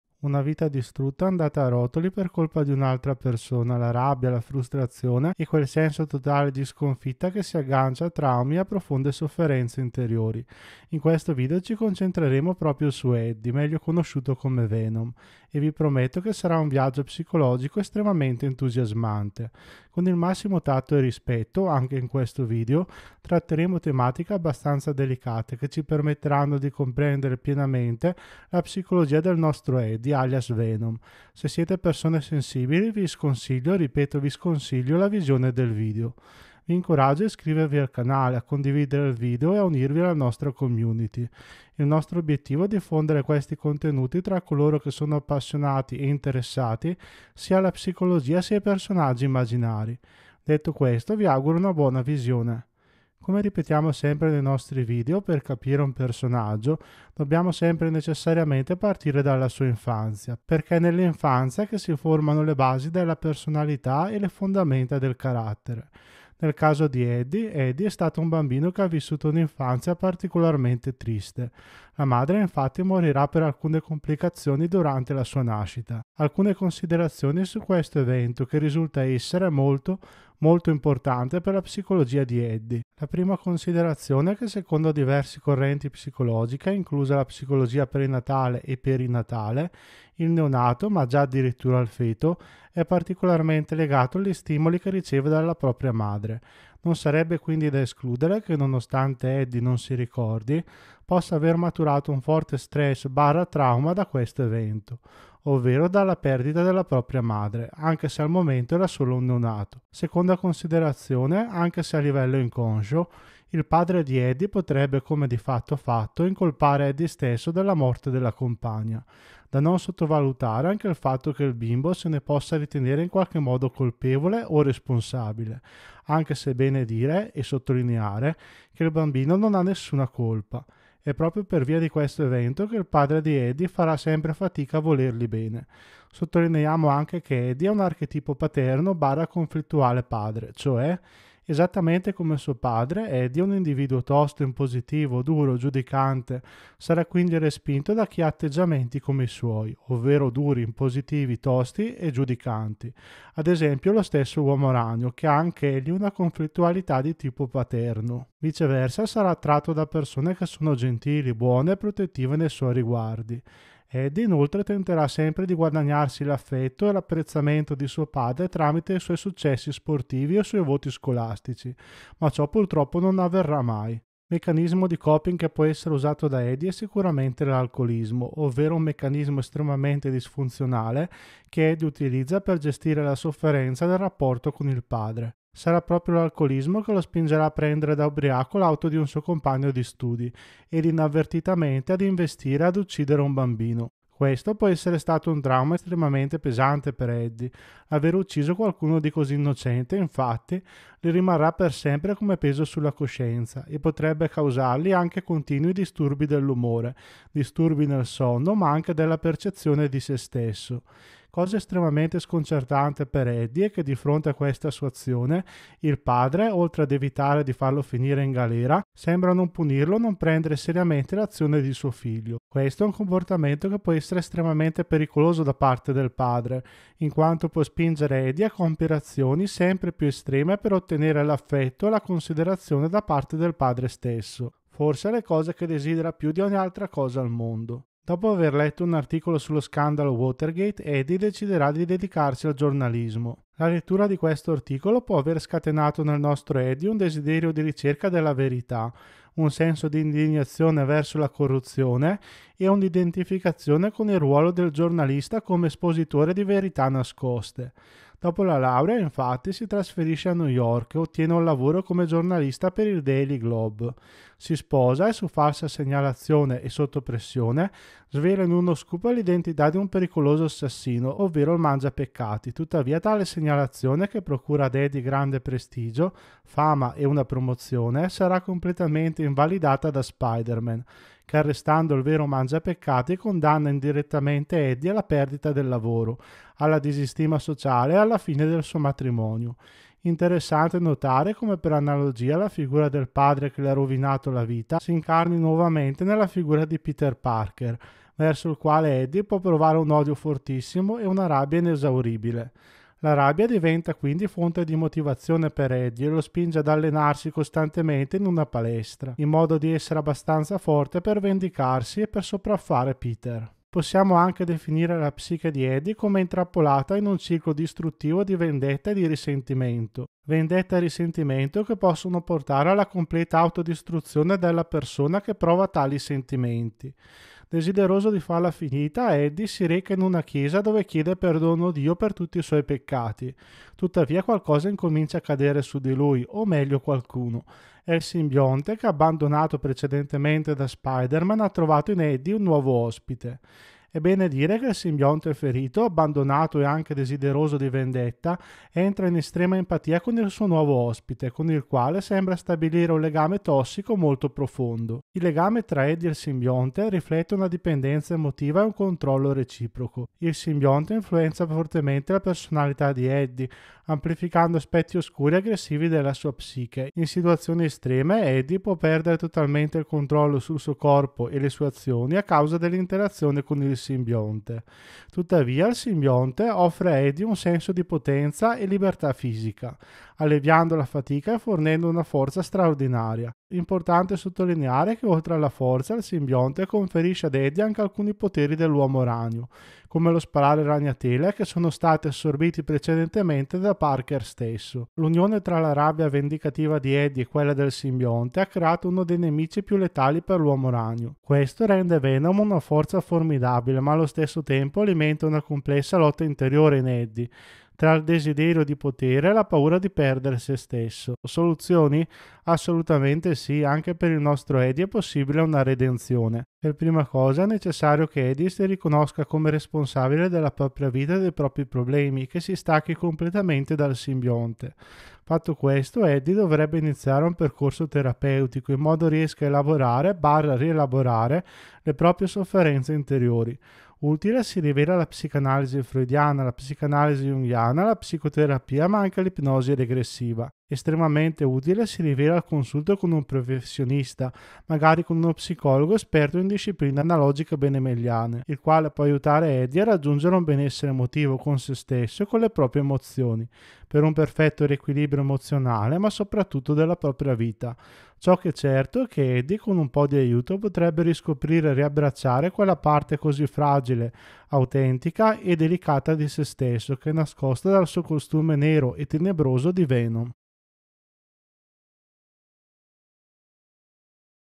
Cedere ai propri demoni, nella mente di Eddie Brock, meglio conosciuto come Venom
Esplora la mente tormentata di Eddie Brock, noto come Venom, in un viaggio avvincente tra traumi, rabbia e redenzione. L'infanzia segnata dalla perdita della madre e un rapporto conflittuale col padre hanno plasmato la sua psiche, portandolo a cercare disperatamente l'affetto paterno. Il giornalismo sembra offrire una via di salvezza, ma l'ingiustizia subita lo condanna a una spirale di vendetta e risentimento. La sua rabbia diventa la forza motrice che lo spinge verso la vendetta contro Peter Parker, incarnazione del padre che gli ha rovinato la vita. Una ricerca ossessiva della verità e un senso di potere lo conducono a un'inesorabile unione con il simbionte, dando vita a Venom, un formidabile nemico dell'Uomo Ragno. Ma questa unione diventa anche una lotta interna tra il desiderio di potere e la paura di perdere sé stesso. Il video offre soluzioni per Eddie, un percorso verso la redenzione attraverso il distacco dal simbionte e un trattamento terapeutico. Unisciti a noi per esplorare le profondità della psicologia di Venom e il suo viaggio verso la ricerca di sé stesso